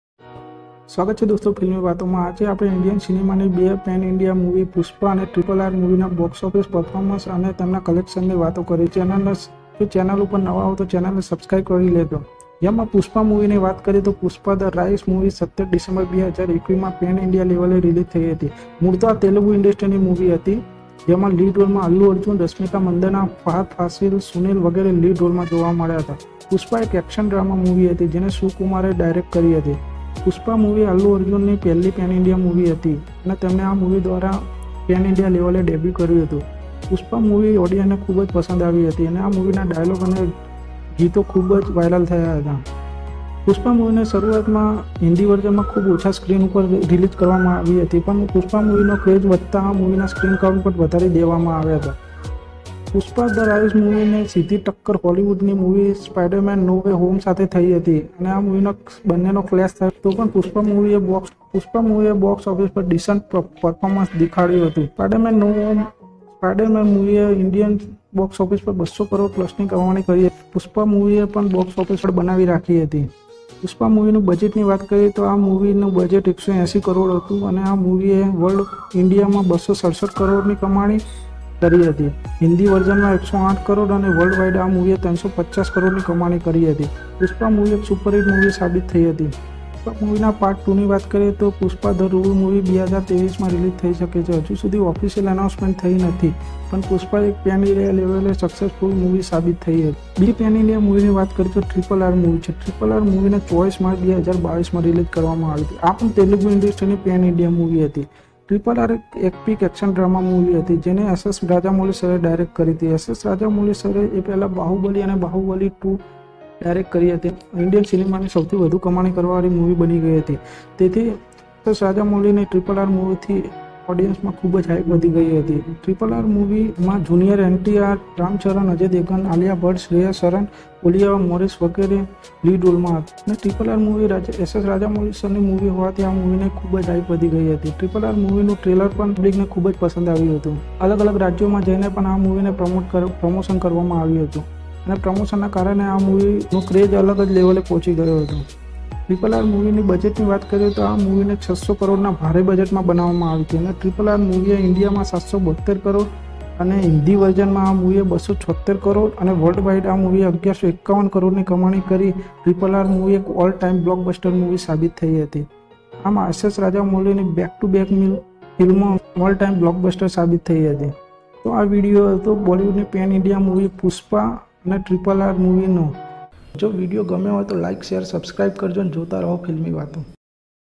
RRR અને Pushpa || Box office Collections
About -
 RRR VS Pushpa Movie Collection
Pushpa- The rise - 17 dec 2021
- Telugu industry Pan India movie
- Allu Arjun, rasmika mandana
  Fahad fashil, Sunil ,
- Action, drama
- Sukumar DIRECT kari hati
- pushpa movie Allu Arjun ni first'
  Pan India movie hati Aa movie
  Thi pan India debut karyu hatu
 Pushpa Audiance khoob pasand
Aavi hati Pushpa Movie dialogue
Song akha India viral thaya
Pushpa movie Hindi version khoob
Screen Apaya hata pan Allu Arjun
Fan Aa movie Hindi version khob
Chali
Biju karan hatu pushpa no clash
Hollywood Marvel ni movie
Spiderman noway home thi thayo
Chata pushpa Spiderman ne Puri
Takkar Aapi hati

 Budget - 180 cr
Collection - 267cr india Net
          Hindi- 108 cr
          W Wide- 350 cr
 Pushpa movie superhit Thai hati
Allu Arjun pushpa the rule movie
2023 release Thai sake che

2) RRR - 24 March 2022
     Telugu industry Pan India ni
   Epic Action drama Movie
   Hati
   RRR Movie ne Bahubali na
 Directar ss Rajamouli sir direct
Kari hati
RRR Movie ma lead role
 - Jr NTR  Ram Charan, ajay devgan
    Alia bhatt, Shreya Saran, Olivia
    Moris hata
 RRR movie ss Rajamouli movie
Hovathi Aa movie mate public
Khoob hype hati Rrr movie ne
India ma badhi Industry ma Jai
North South badhi jagat RRR Movie team dwara pramotion
Karvama Avyu hatu RRR Movie
Trailer pan khoob pasand karavana avyu hatu

RRR movie nu Budget - 600cr
  Collection - India 772 cr
                       Hindi - 276 cr
                       W Wide-1151 cr
  Verdict - All Time Blockbuster